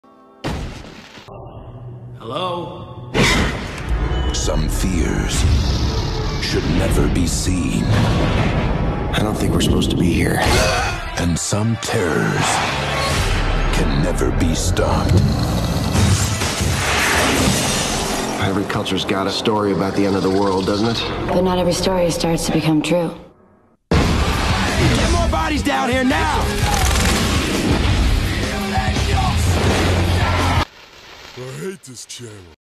Patrick Hates Alone in The Dark (2005)
Patrick hates one of the worst films ever been made of 2005